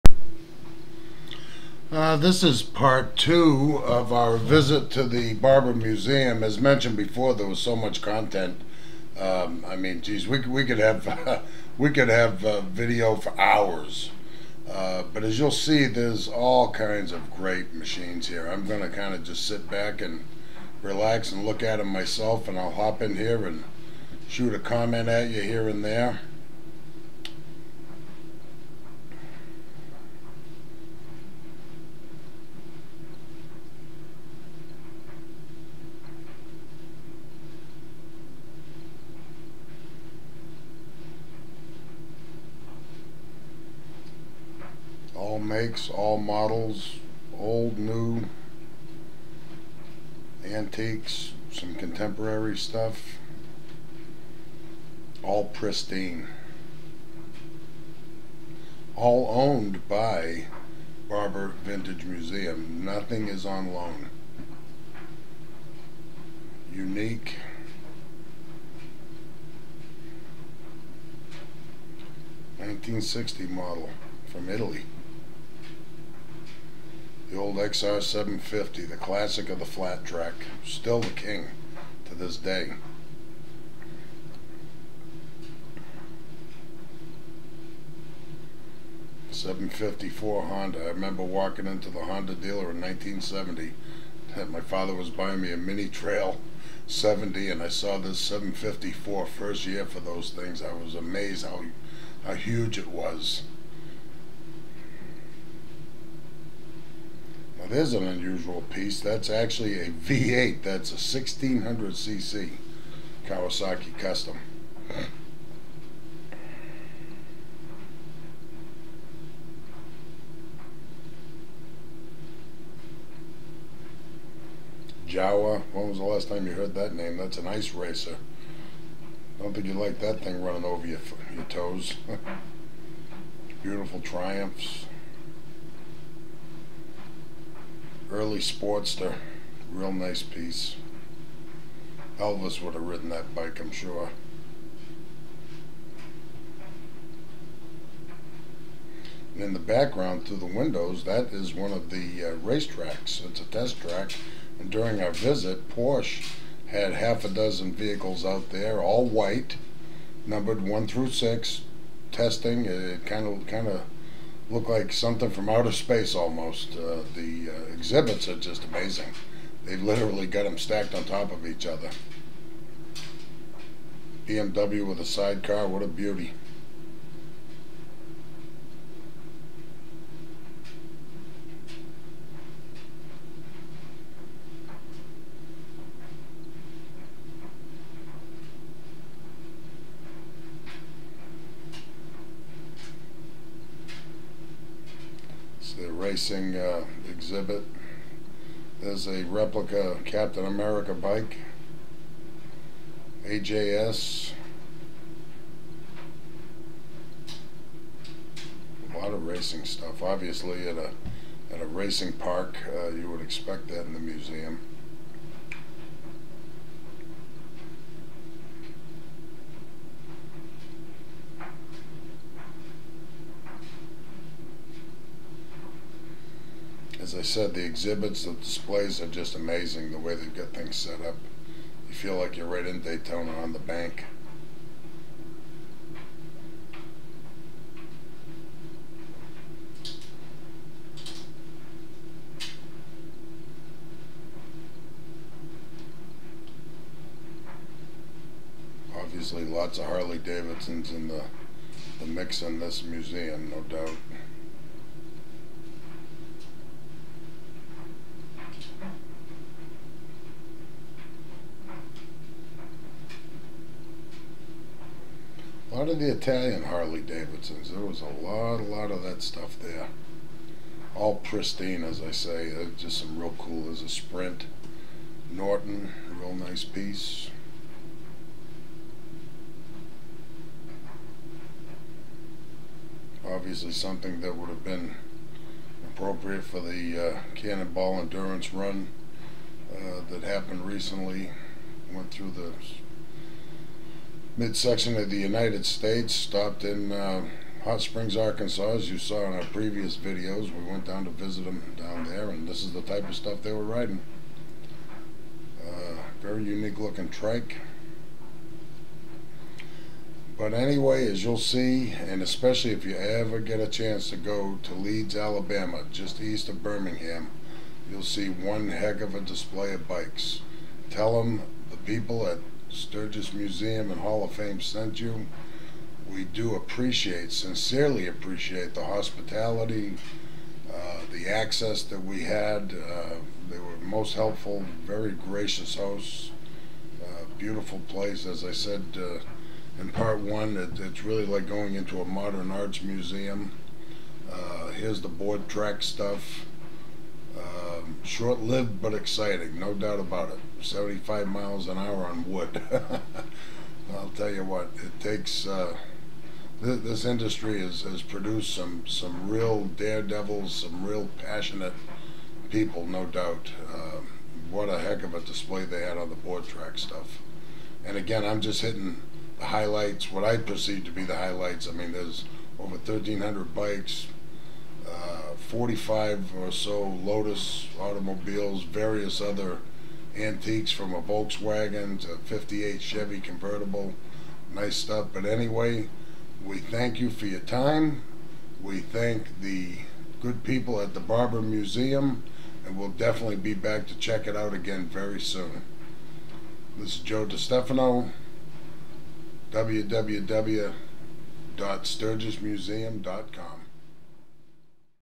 Barber Museum part 2
Sturgis Motorcycle Museum visits the Barber Museum in Leeds, Alabama